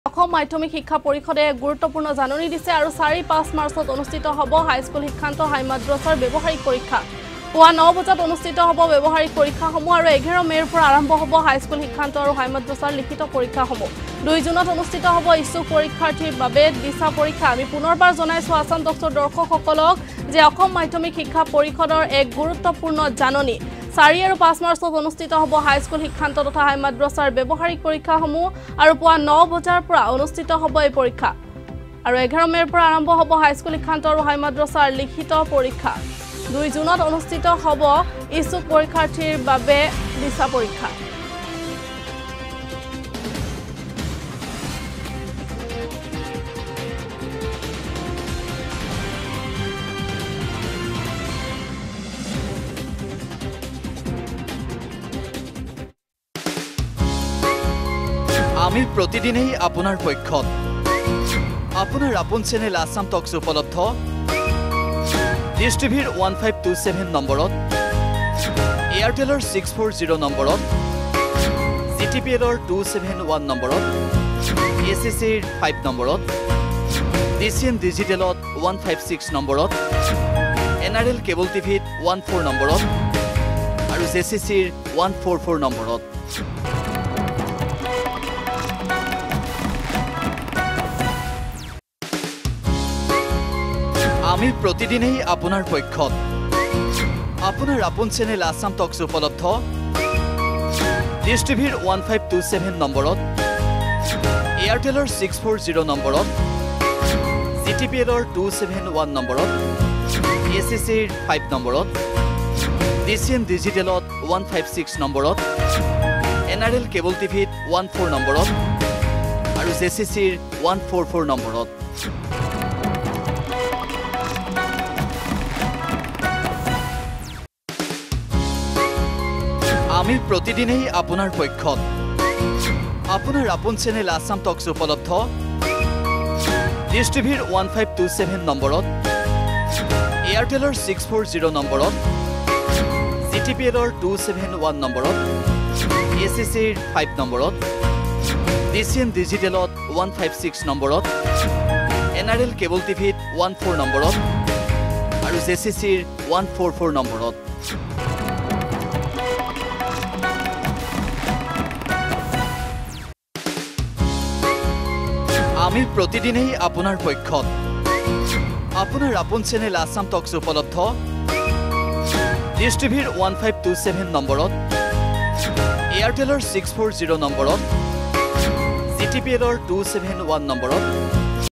৪,৫ মাৰ্চত অনুষ্ঠিত হ’ব হাইস্কুল শিক্ষান্ত/হাইমাদ্ৰাছাৰ ব্যৱহাৰিক পৰীক্ষা।
৪,৫ মাৰ্চত অনুষ্ঠিত হ’ব হাইস্কুল শিক্ষান্ত/হাইমাদ্ৰাছাৰ ব্যৱহাৰিক পৰীক্ষা। ১১মে’ৰ পৰা আৰম্ভ হ’ব হাইস্কুল শিক্ষান্ত আৰু হাইমাদ্ৰাছাৰ লিখিত পৰীক্ষাসমূহ।